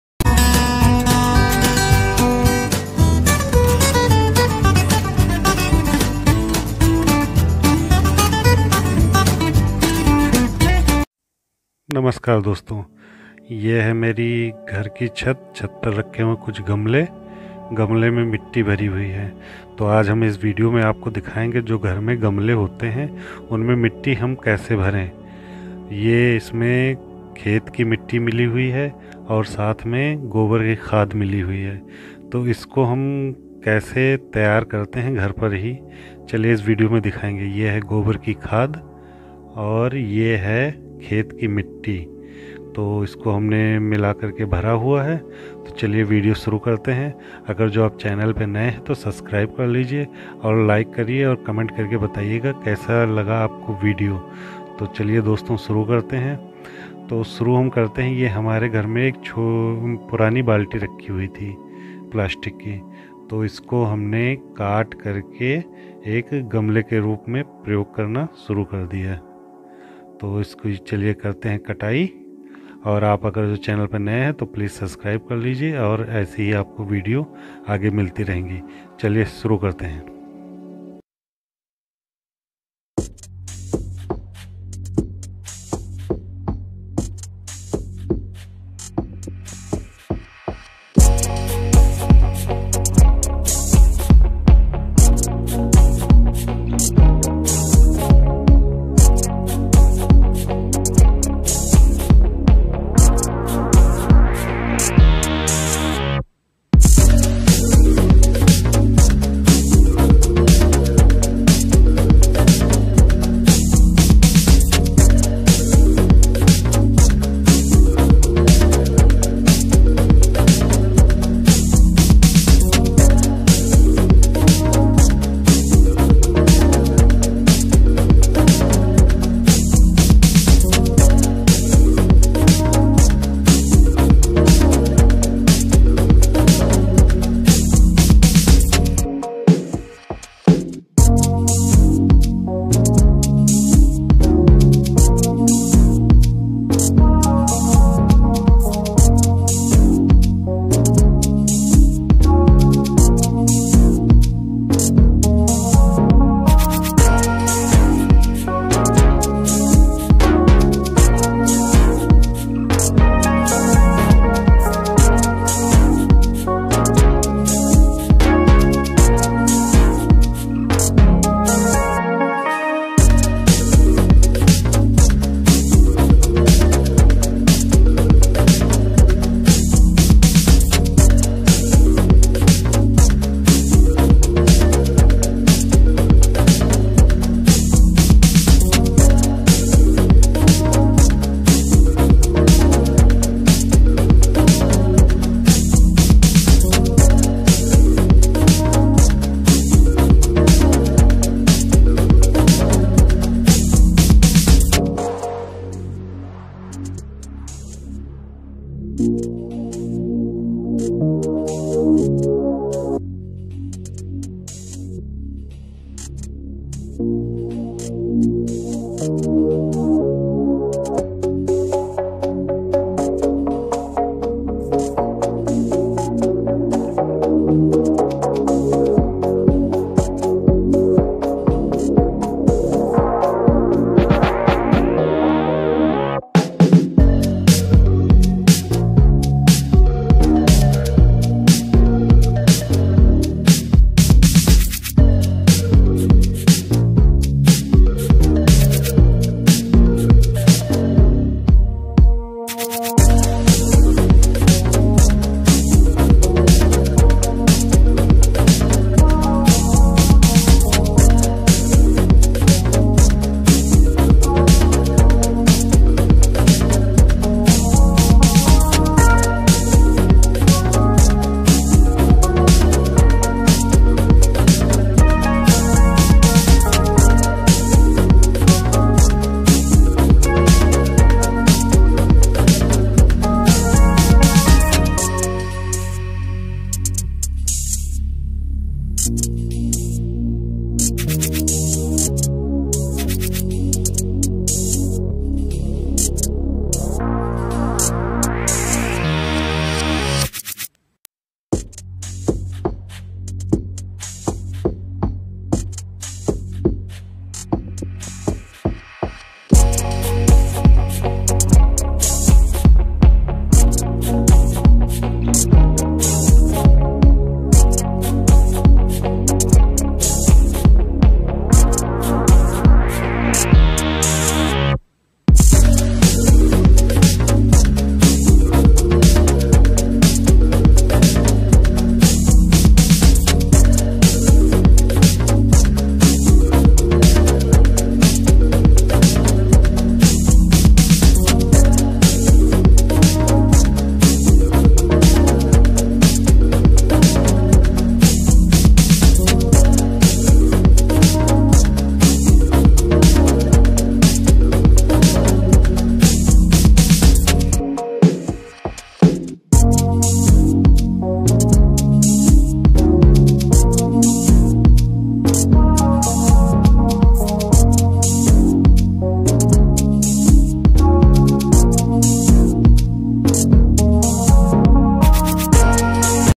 How prepare organic compost for pot/गमले के लिए आर्गेनिक खाद कैसे तैयार करे /Gobar ki khad
नमस्कार दोस्तो आप देख रहे है :-Fun With Birju
आज की इस वीडियो मे आप देखेंगे कि हम घर पर अपने गमलों के लिए गोबर की खाद कैसे तैयार करते है ।

In today's video you will see how we prepare cow dung manure for our pots at home.